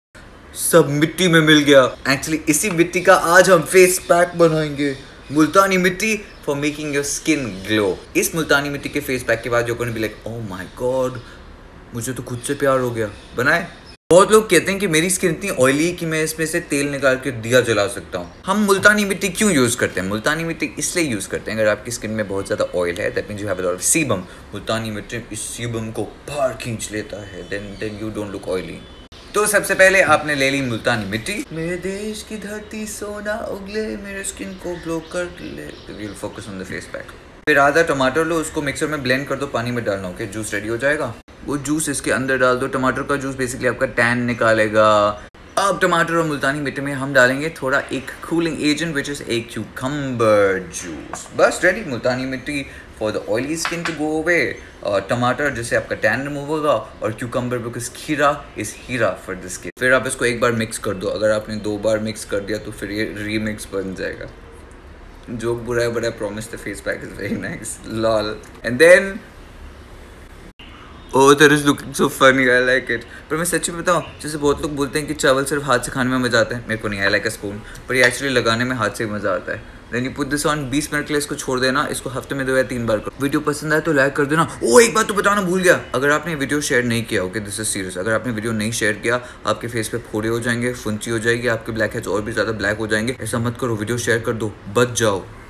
Oily skin?! Try This!
Multani mitti, when mixed with these two ingredients will help remove excess oil, get rid of a tan and cool down your skin!